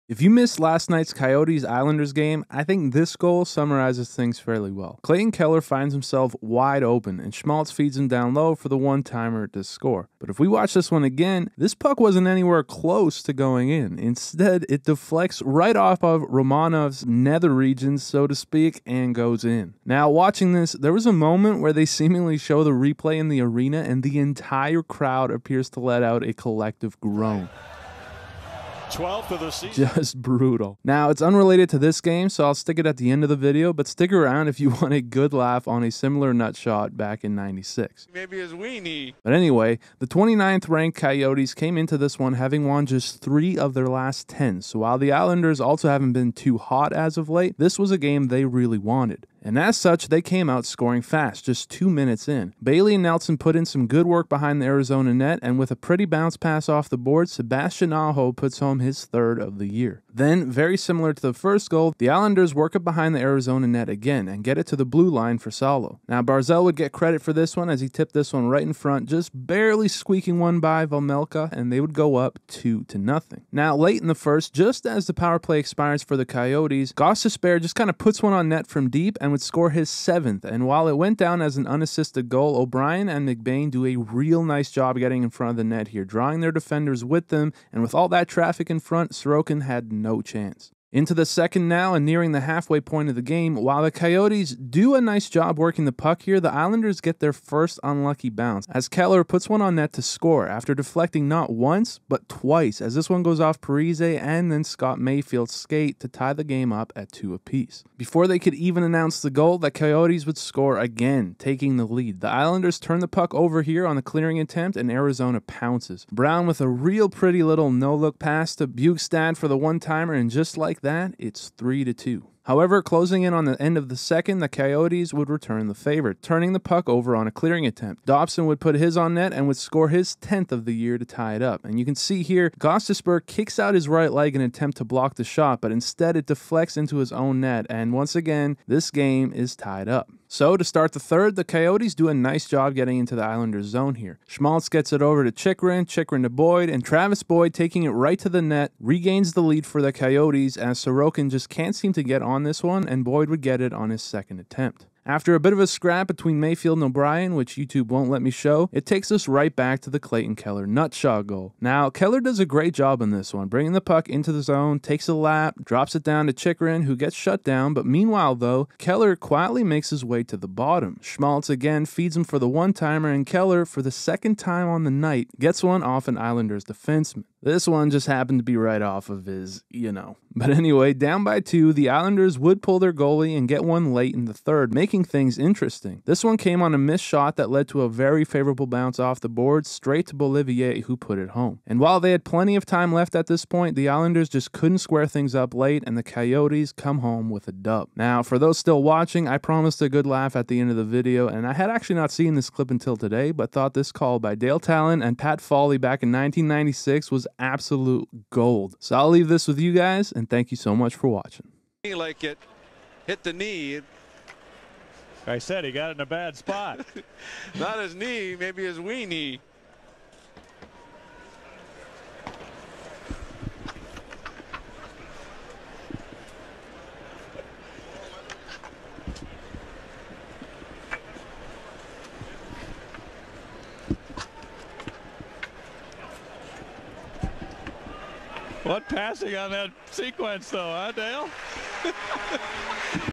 Keller scores SLAPPER off of Romanov's nuts... 😬
Ooof Keller scored a slap shot goal off of Romanov's nuts in last nights Arizona Coyotes vs New York Islanders game. The Islanders came into this one hoping to turn things around at this point in the season on a 29th ranked Coyotes team that was just 3 and 7 in their last 10. I cover the "nut shot" and everything else in todays NHL hockey video recap!